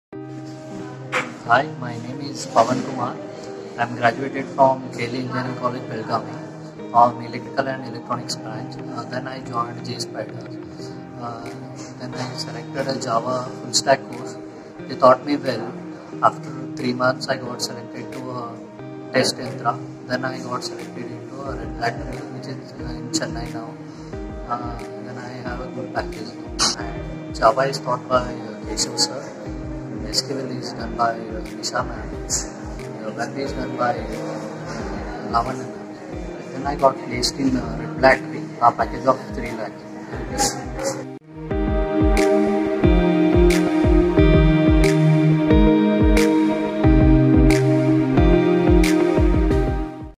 Jspiders Basavanagudi | Student Testimonial | Placed as DEVELOPER
Congratulations PAVANKUMAR PANCHAL for getting placed as DEVELOPER